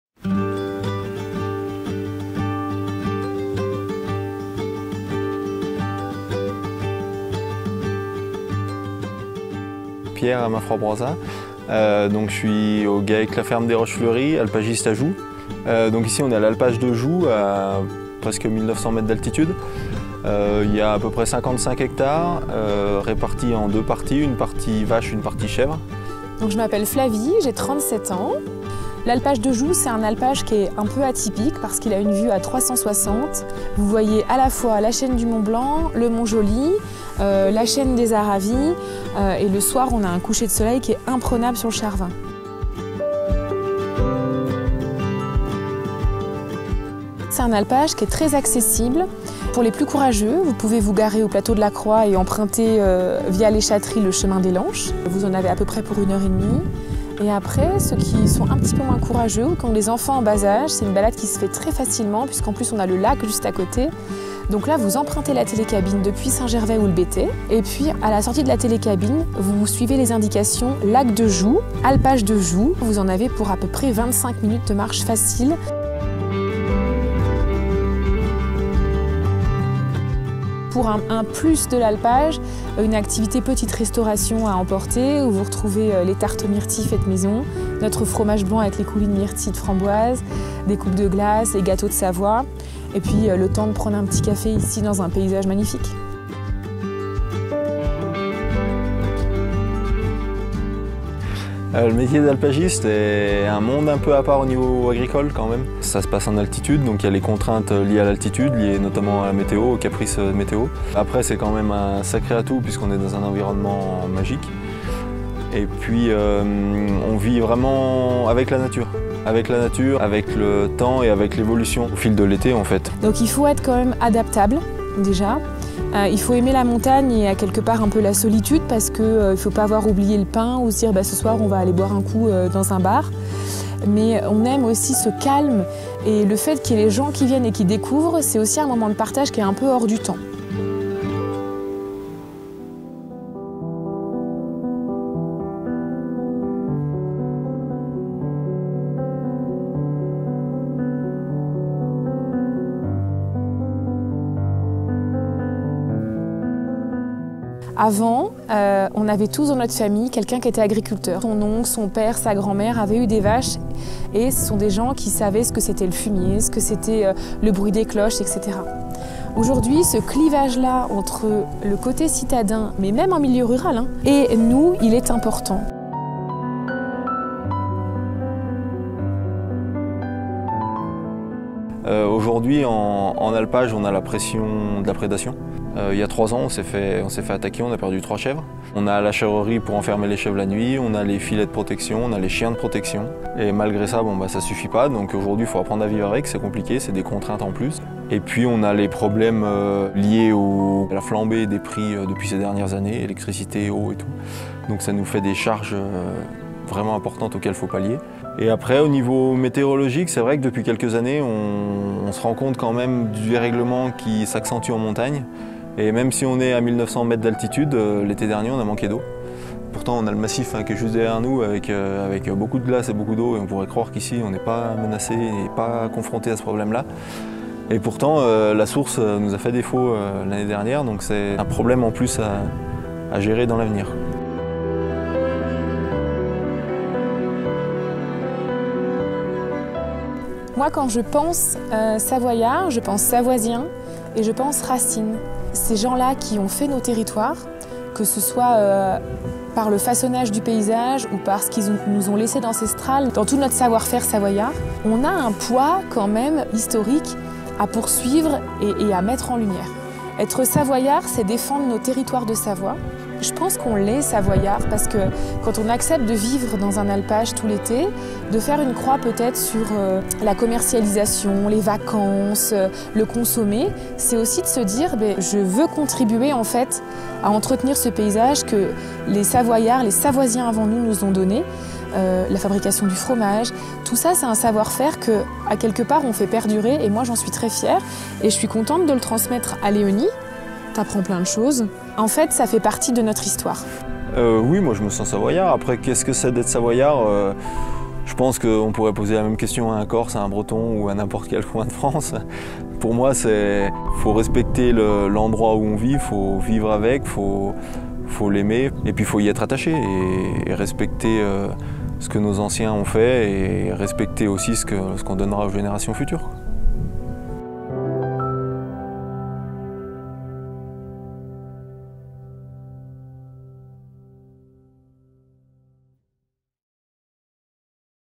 Entre lac et montagne, découvrez l'alpage de Joux
Rendez-vous en alpage – Épisode 4 : L'alpage de Joux

L'alpage de Joux est situé sur le domaine skiable de Saint-Gervais, sous le Mont Joux, à la frontière avec le village de Megève. Niché à 1900 mètres d'altitude au bord d'un lac, cet alpage de montagne bénéficie d'une vue spectaculaire à 360°, du Mont-Blanc au Aravis en passant par le Mont-Joly ! 😍

Fabrication de fromage, traite des animaux, gastronomie de montagne et dégustation de produits locaux, à l'occasion d'une randonnée dans les hauteurs de Saint-Gervais Mont-Blanc, découvrez l'alpage de Joux et les vaches, cochons ou chèvres qui profitent de leur estive dans un cadre naturel époustouflant. 🐮🏔